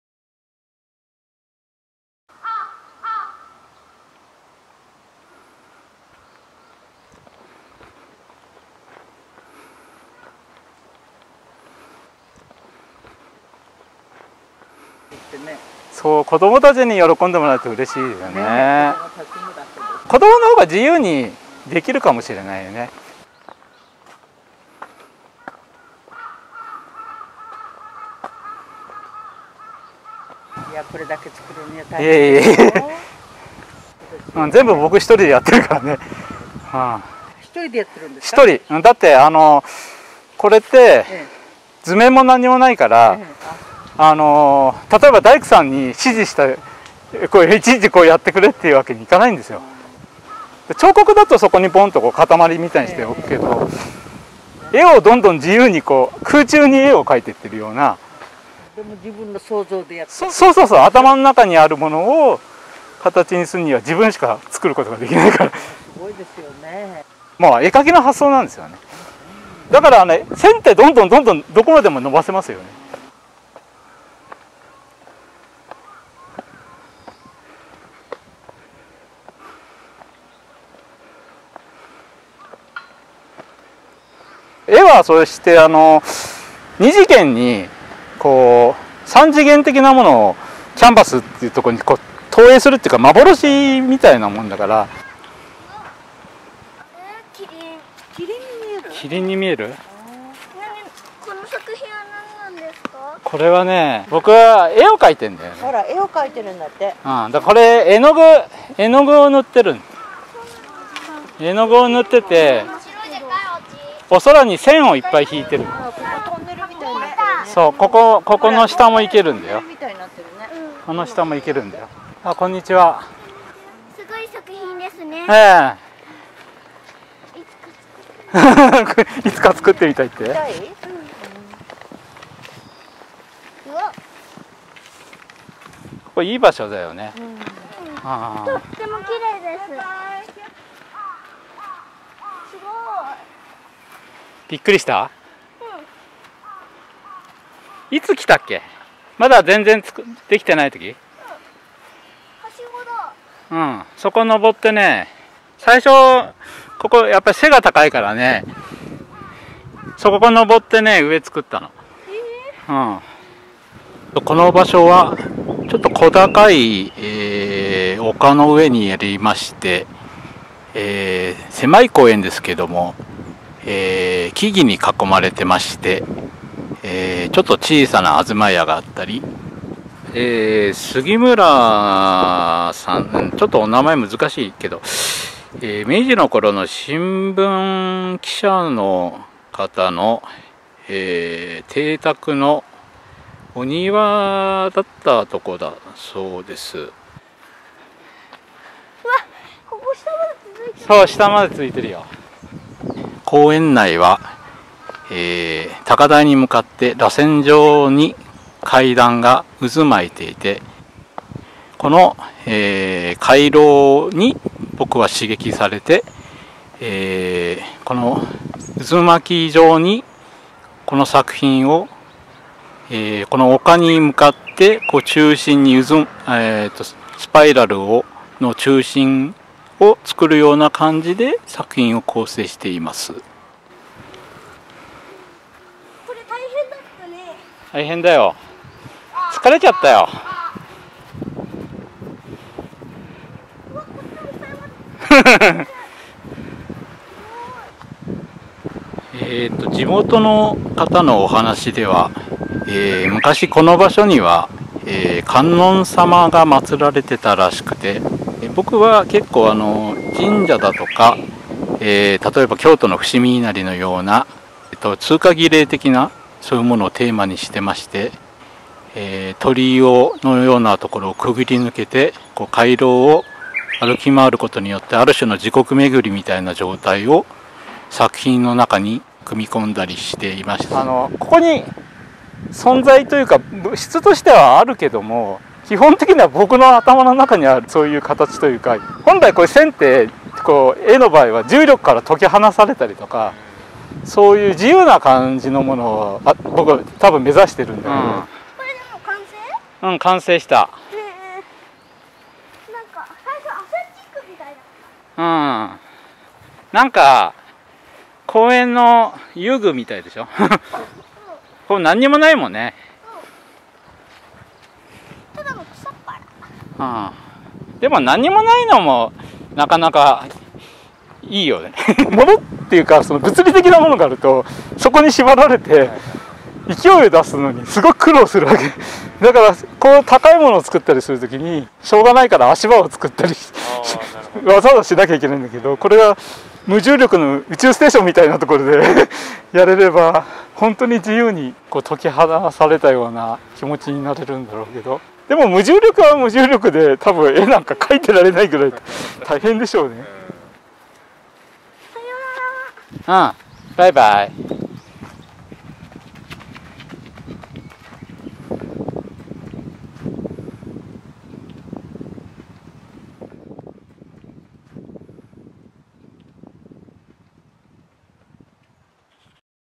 中村岳作品 ドキュメンタリー映像 楚人冠公園【我孫子アートな散歩市第20回記念】 ABIKO ART Wailk 2021年5月8日(土)〜6月6日(日)
中村岳作品。我孫子での記録映像を作りました。ぼくの制作では、地元住民との会話が重要です。子供との交流が可愛いです。Nakamura Takeshi's work in ABIKO,Japan. I made a documentary video. English subtitles. Conversation with the locals is important in my production. The interaction with children is cute.